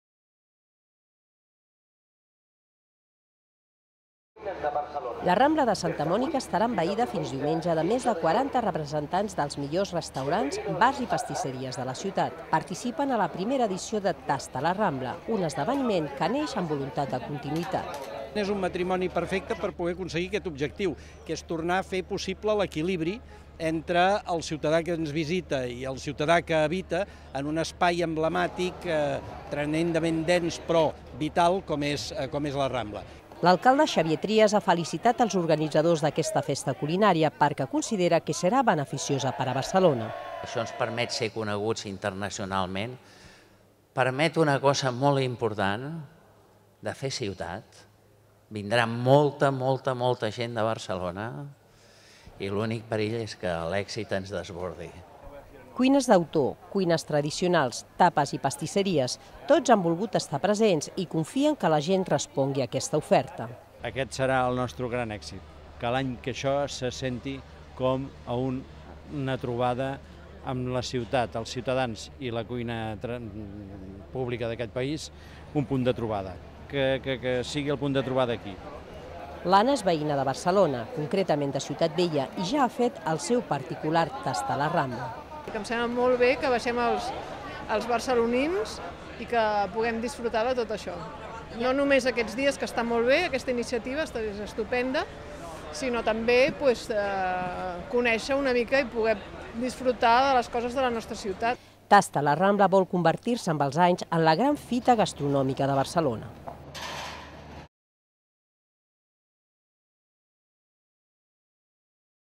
Degusta Barcelona al Tast a la Rambla
La Rambla de Santa Mònica es converteix fins diumenge vinent en l'escenari de 'Tast a la Rambla', el primer festival culinari de Barcelona. Aquest esdeveniment, que neix amb voluntat de continuïtat, aplega 42 dels millors restaurants, bars i pastisseries de la ciutat. Més informació